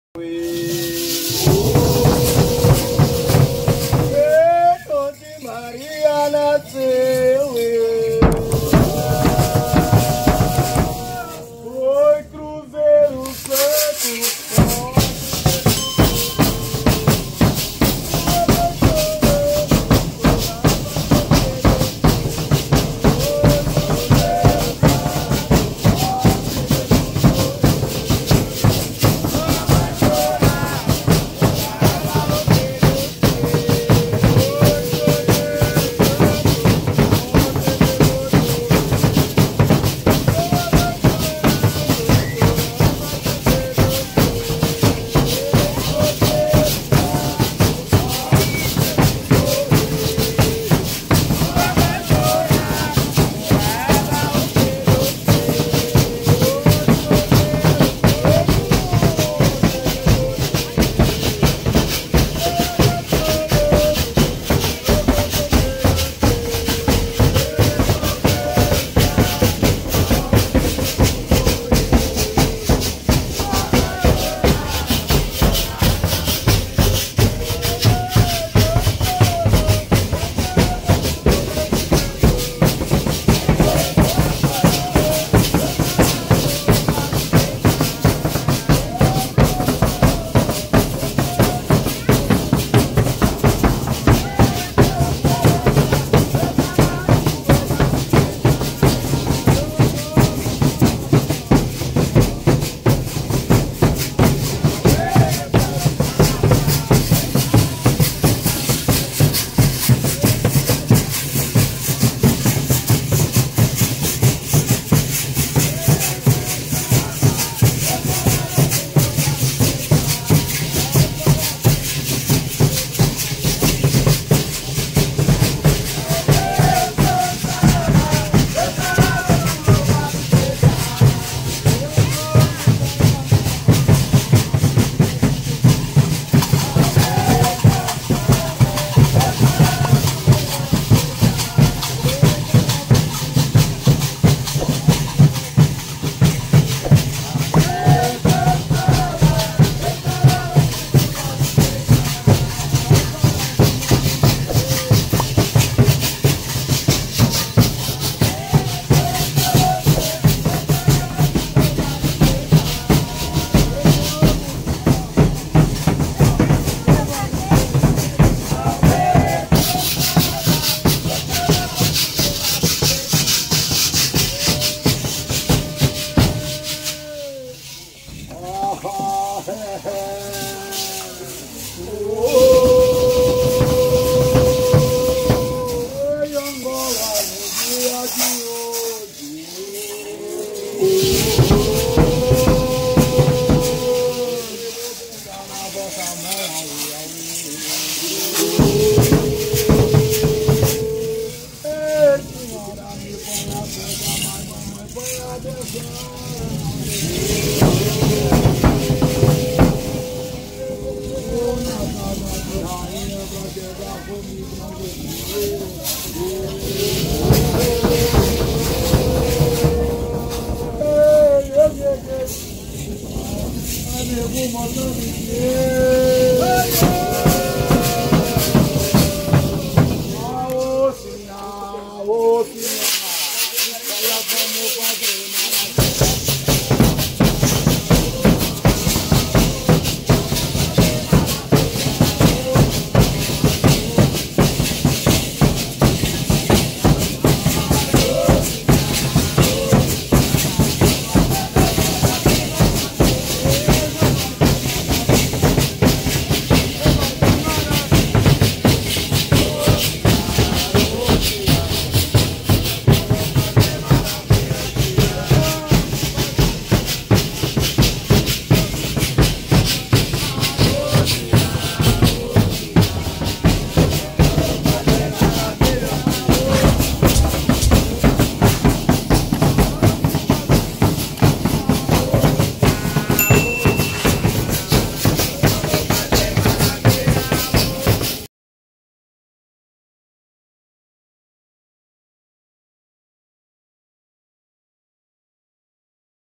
Reinado Em Itaúna MG 2022, Moçambique Santa Efigénia De Itaúna MG, Casa Da Rainha!
Olá Pessoal!
Veja Que Aconteceu Neste Domingo De Festa Em Itaguara Mg!
Fui Convidado A Participar Desta Linda Festa De Reinado!
Festa De Tradição Na Região Com Décadas De História!
Fui Bem Recebido Por Todos Os Congadeiros Reis E Rainhas Em ITAGUARA!
E Também Em Todas As Festas Que Estive Presente Em Outras Cidades!
Com Várias Guardas De Outras Cidades Fizeram Uma Linda Festa!
Com O Reino Coroado E Guardas Puxando Coroas De Promessas Em Devoção A Nossa Senhora Do Rosário!
Festa Linda Mais Um Domingo Abençoado.
SALVE MARIA MEUS IRMÃOS DE ROSÁRIO!
VIVA O ROSÁRIO!